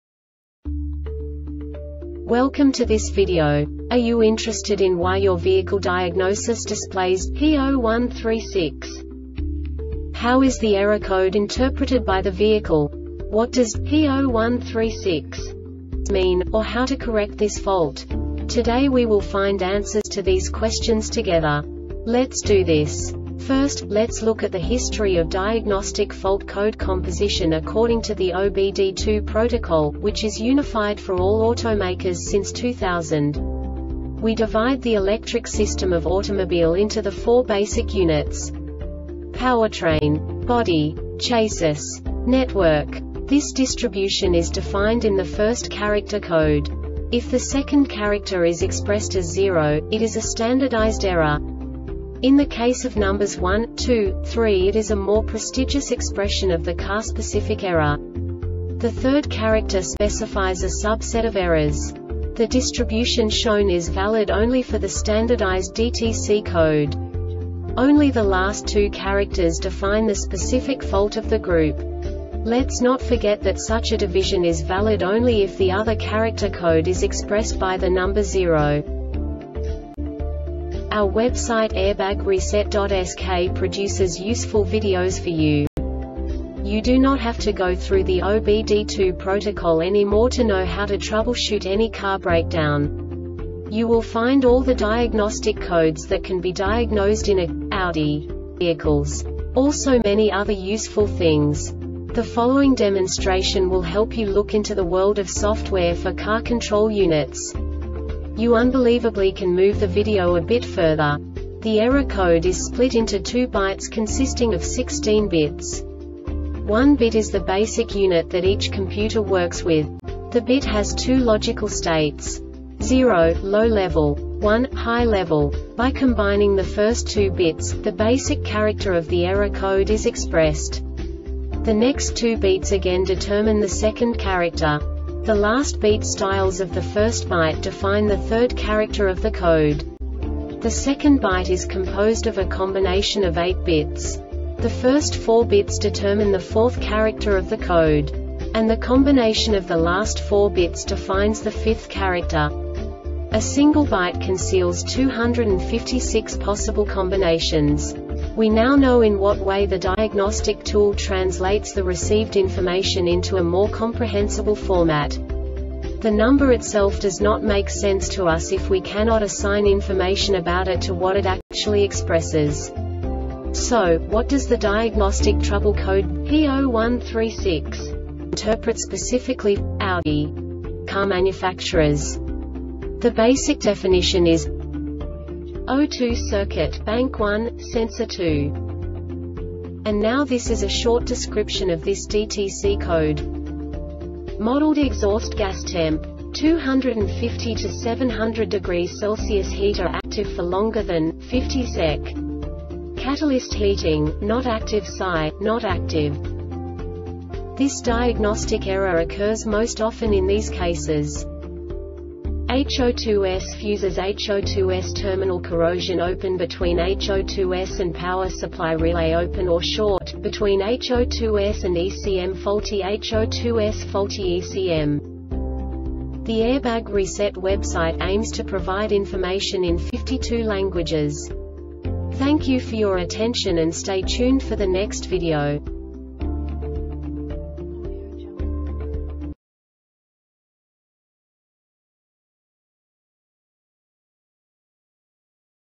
DTC Audi P0136 Short Explanation
Contents:
0:21 Basic DTC analysis according to OBD2 protocol standard.
1:48 Insight into programming
2:58 A diagnostically understandable description of the Diagnostic Trouble Code
3:05 Basic error definition

Make:
Audi
Code:
P0136
Definition:
O2 Circuit (Bank 1, Sensor 2)
Description:
Modeled exhaust gas temp. 250-700 degree  C Heater active for longer than, 50 Sec. Catalyst heating, Not active SAI, Not active
Cause:
HO2S Fuses
HO2S terminal corrosion
Open between HO2S and Power supply relay
Open or Short? between HO2S and ECM
Faulty HO2S
Faulty ECM